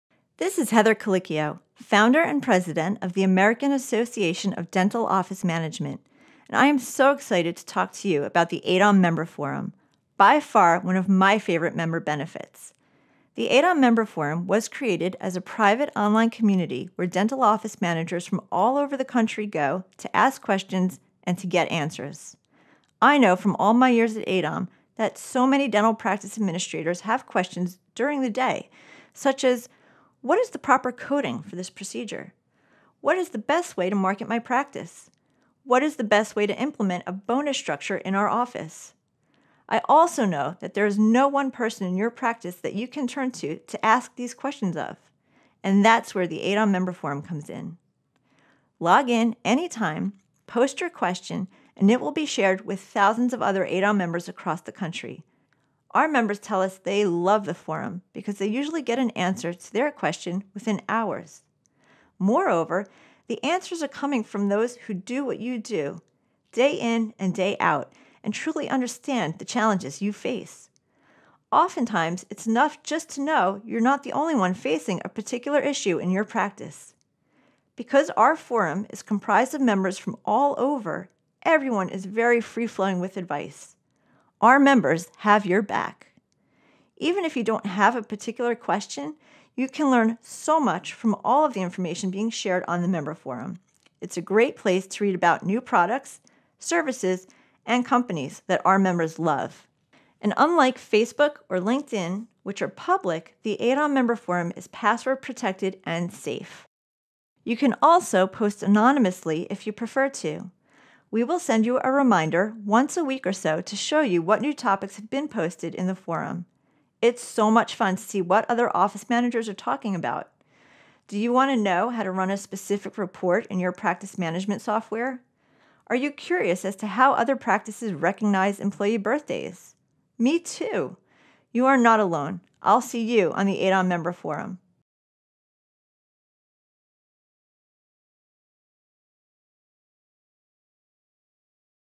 AADOM Orientation - Members Only Forum
Our AADOM Orientation Series helps you understand and utilize all of the benefits of AADOM. Learn more about one of our most valued benefits, the confidential Members Only Online Forum.